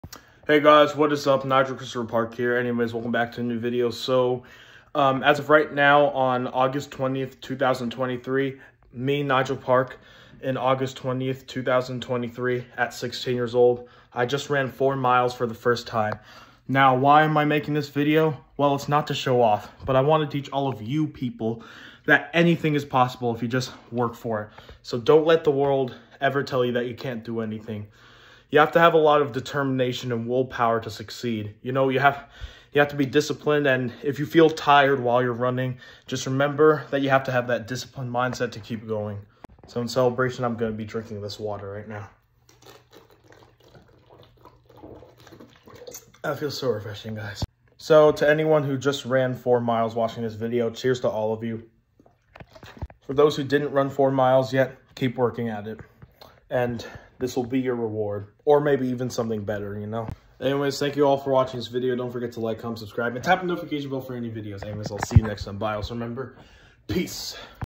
I just ran 4 miles for the first time today
Remember don’t ever let the world tell you what you can’t do, Jesus loves all of you, and he wants you all to succeed Peace!